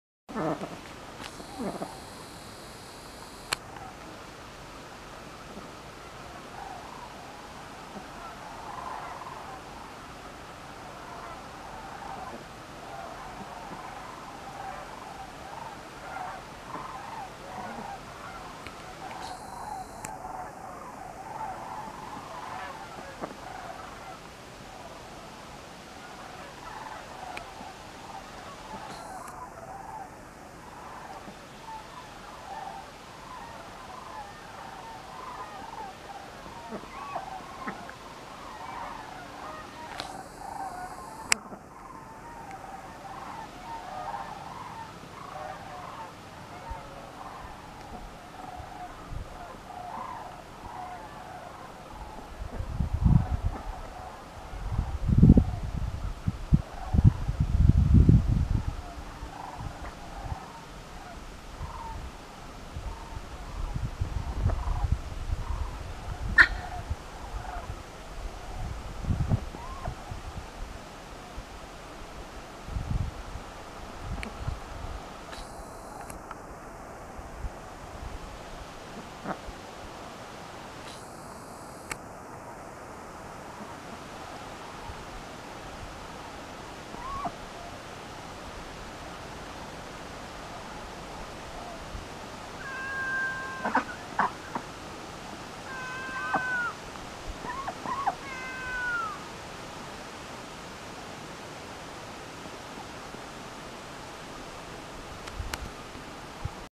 Sandhill Cranes - October 11, 2017
I was standing in my ducks' pen when I saw a large flock of Sandhill Cranes flying overhead.